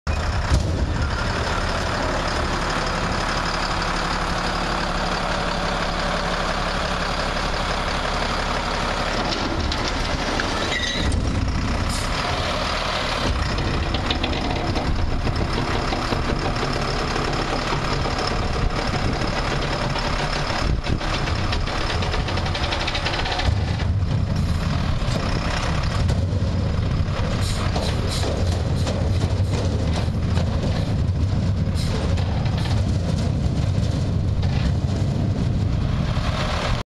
MEGA (M.T.G.) Disposal Truck 67
One of three recycling trucks handed over by the Barrington DPW, Truck 67 is the oldest (and rustiest) truck owned by MEGA.